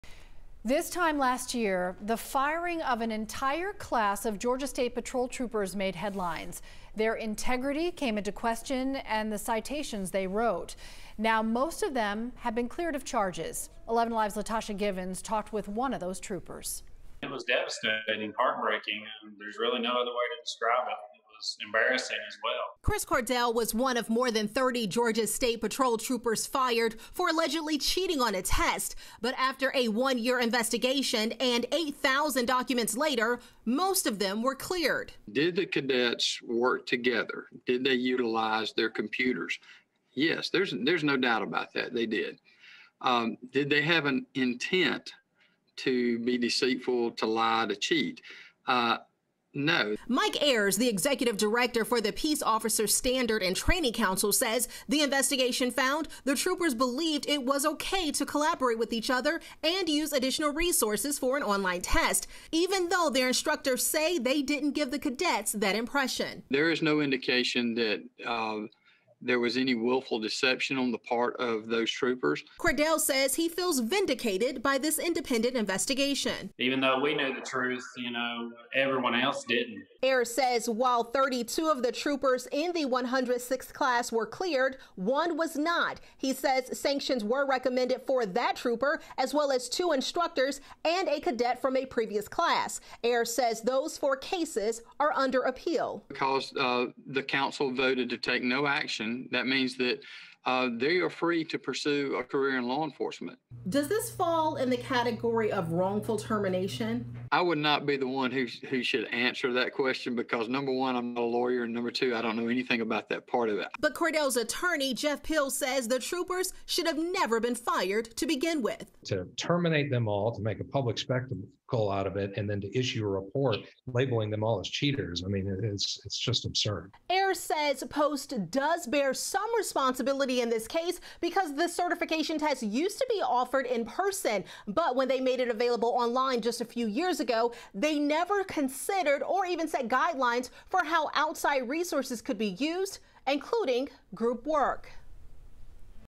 Many Georgia State Patrol troopers accused in cheating scandal cleared of charges
One year and 8,000 documents later, most of the more than 30 accused troopers have been cleared of charges.